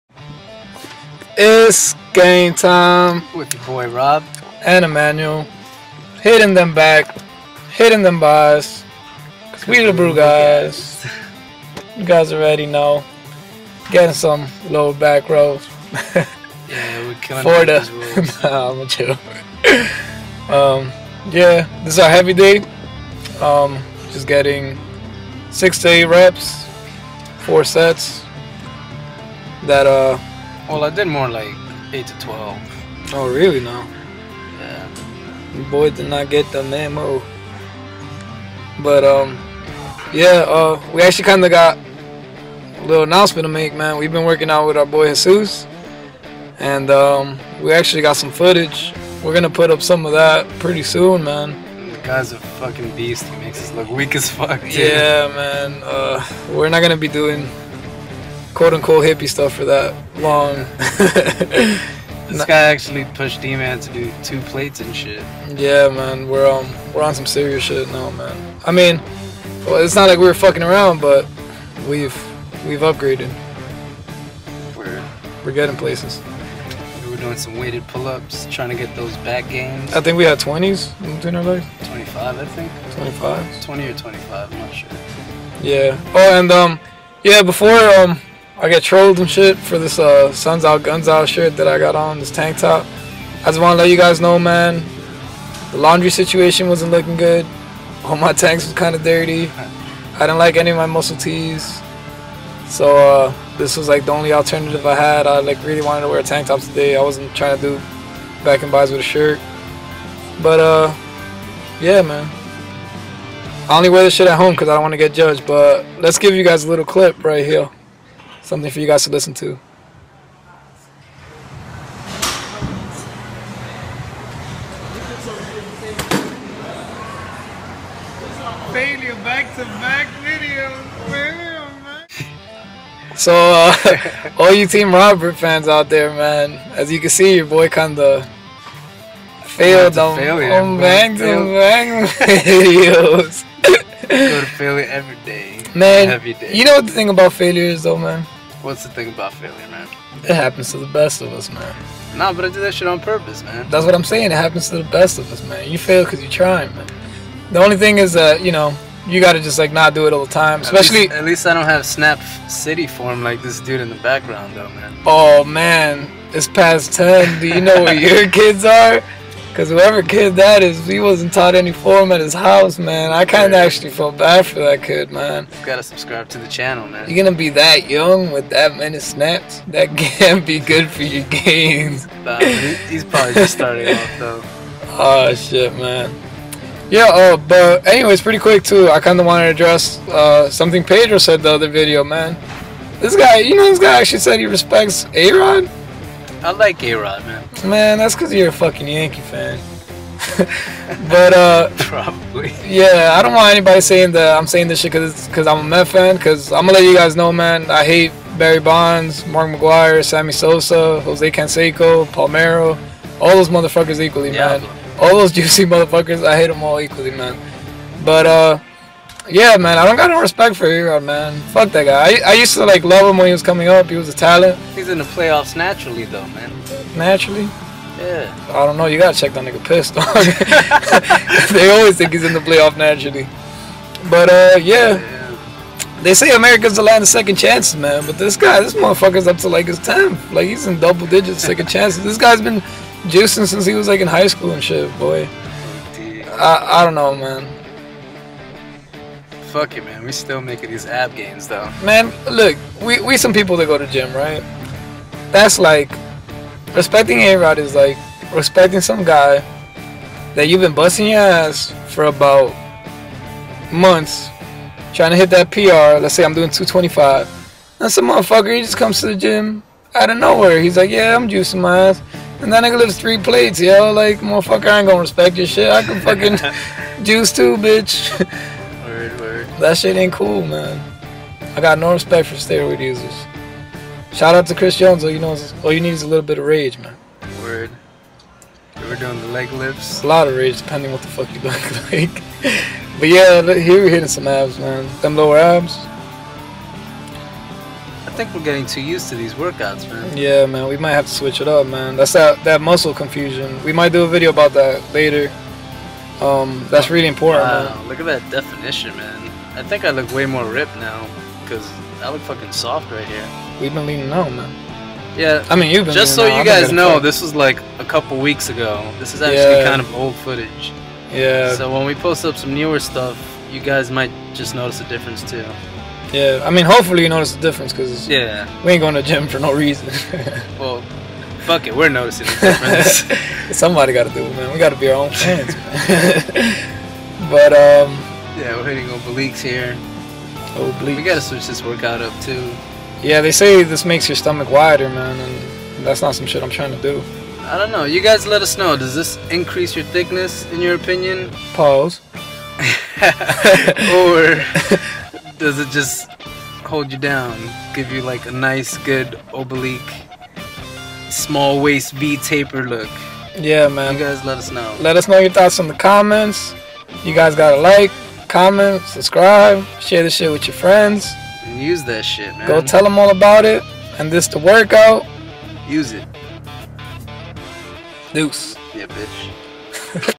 Back and Biceps Work-Out (Heavy Day)
ITS GAIN-TIME! Emmanuel and Robert hit some back and bis for some strength training while cracking jokes and avoiding snaps.

Todays Work-Out (Heavy days consist of 4 sets, 6-8 reps)
Lower back rows
Lat Pull-Downs
Pull-Ups
Biceps Curls
Hammer Curls
Shrugs
Overhead curls

Abs Work Out
-Heavy abs day we typically use weight and go for 12-15 reps
Machine Crunches
Leg Lifts
Oblique Crunches